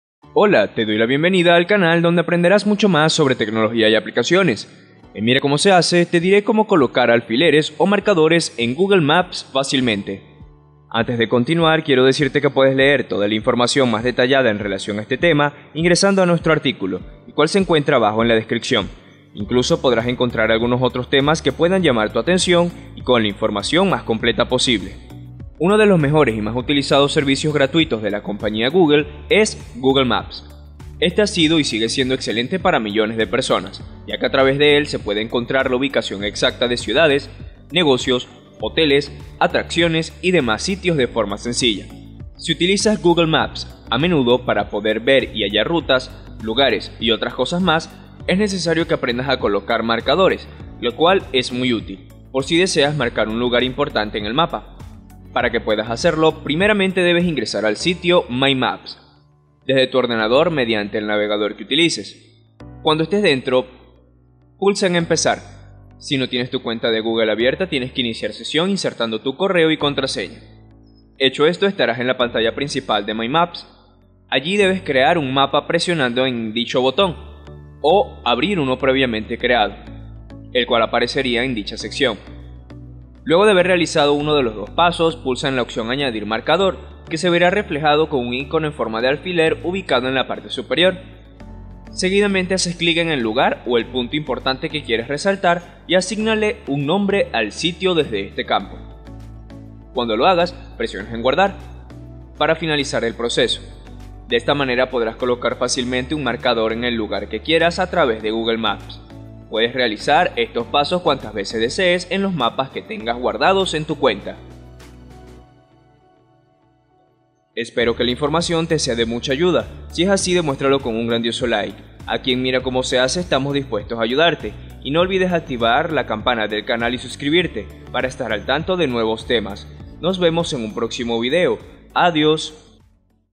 Cómo Colocar Alfileres o Marcadores en Google Maps Fácilmente
En este video te mostramos Cómo Colocar Alfileres o Marcadores en Google Maps Fácilmente. En este canal encontrarás los mejores tutoriales. ¡No te lo pierdas!